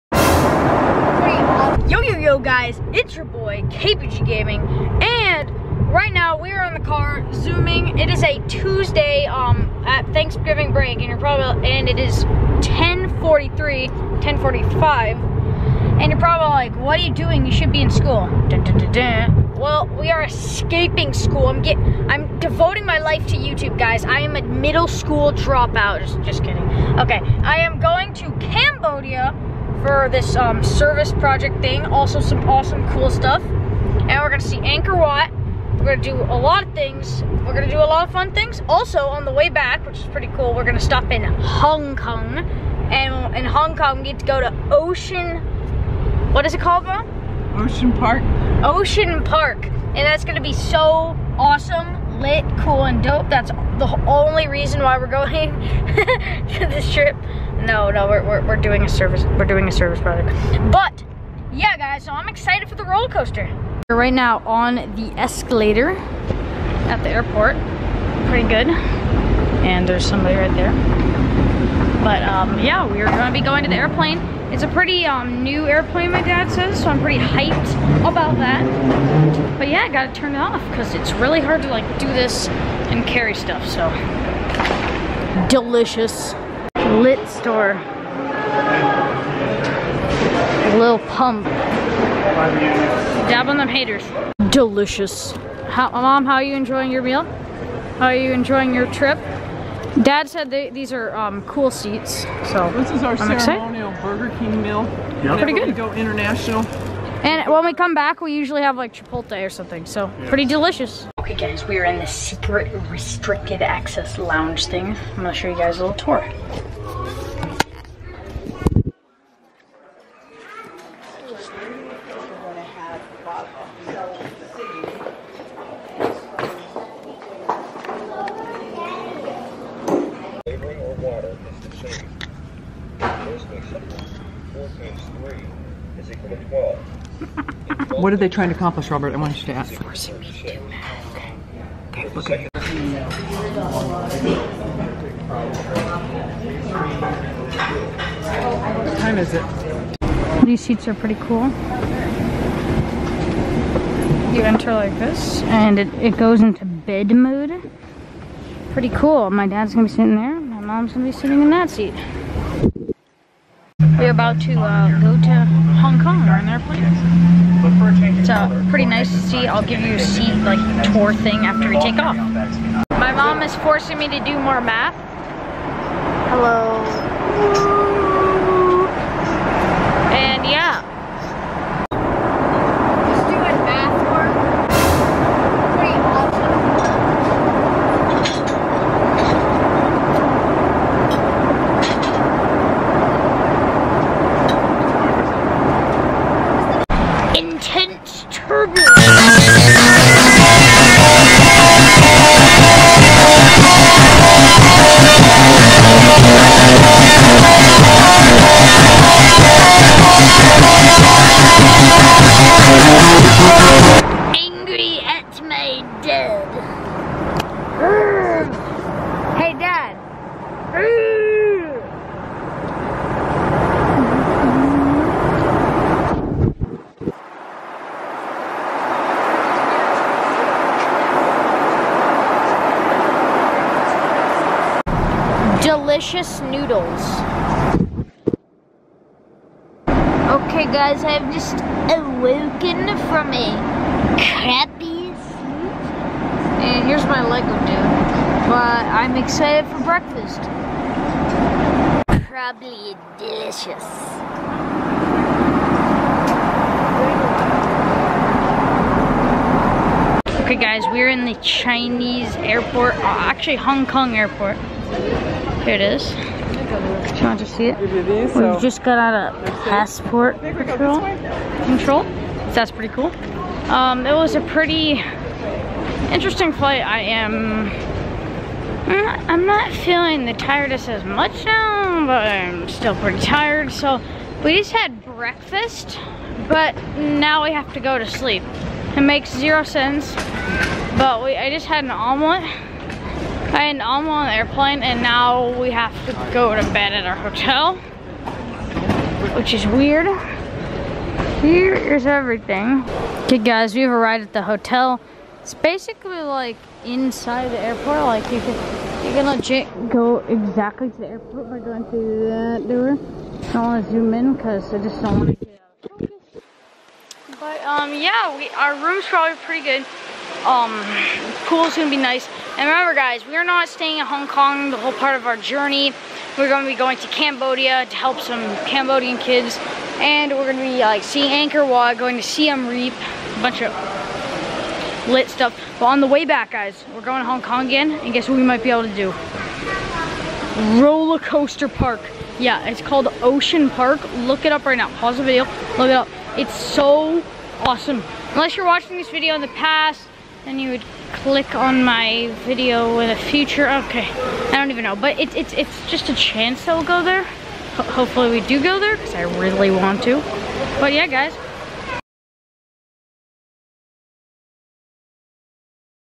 WE PAID THIS MUCH FOR THESE SEATS.......
We are going to Cambodia. In this video WE PAID THIS MUCH FOR THESE SEATS we will be flying in First Class seats to HongKong. But guys WE PAID THIS MUCH FOR THESE SEATS and this amount is a lot.